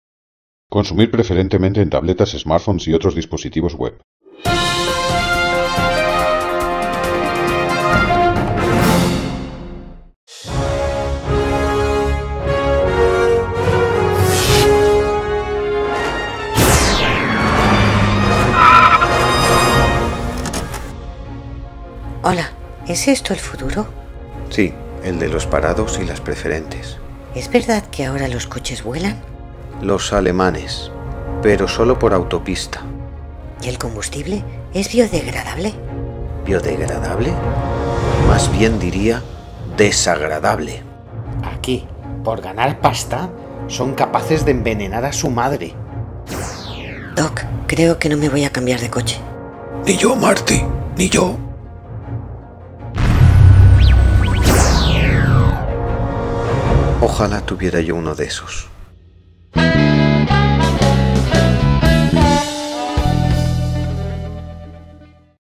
El coche del futuro
Chascavídeo del por qué Marty McFly y Doc Brown no se compraron un Volkswagen al regresar al futuro.